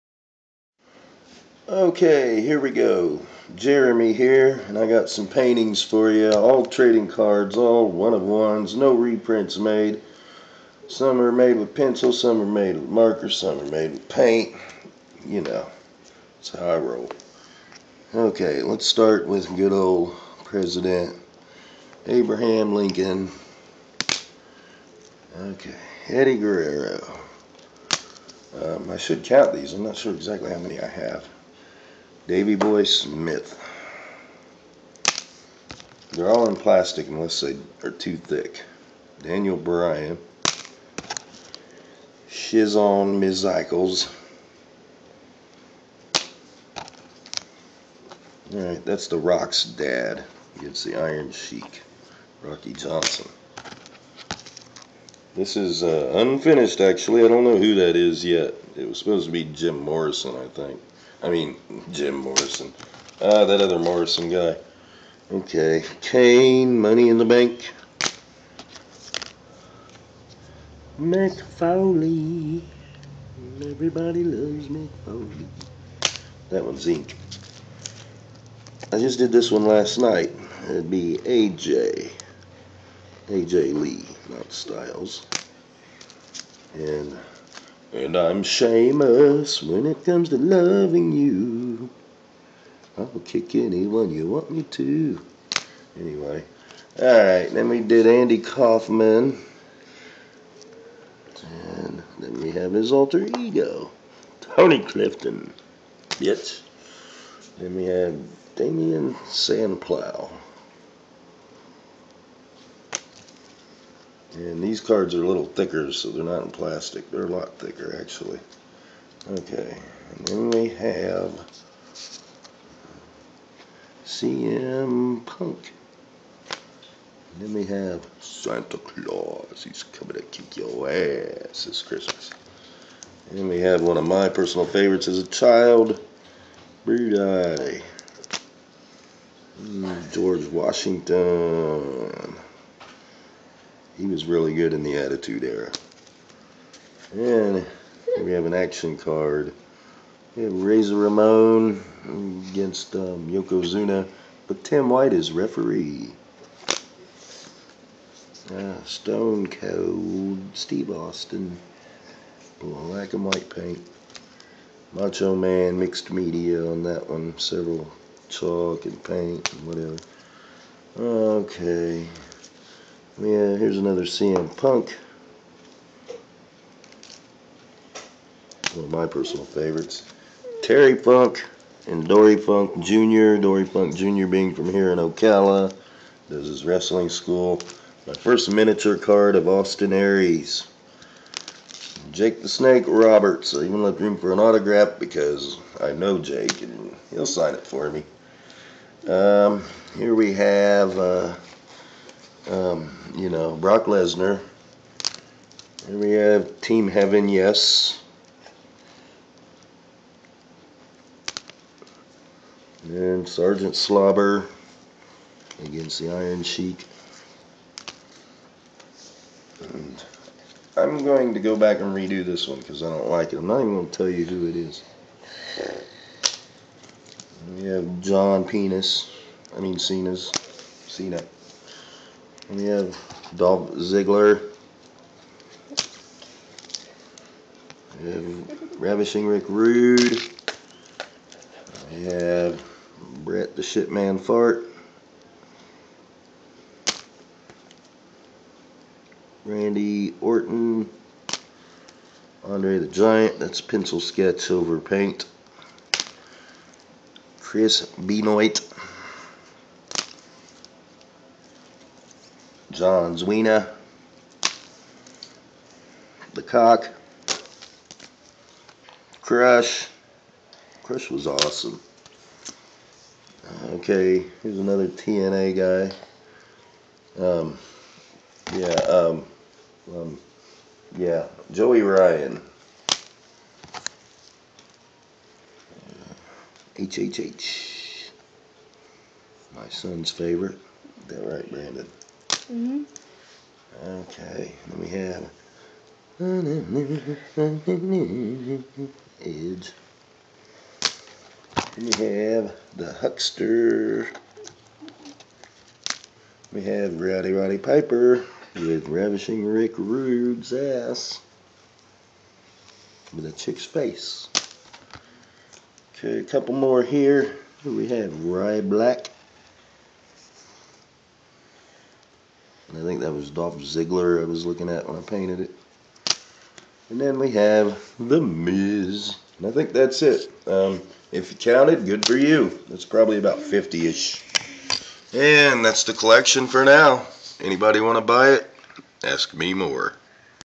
45 art cards. All original art by Jeromy Taylor
Many miniature paintings of professional wrestlers. Entire collection up to date!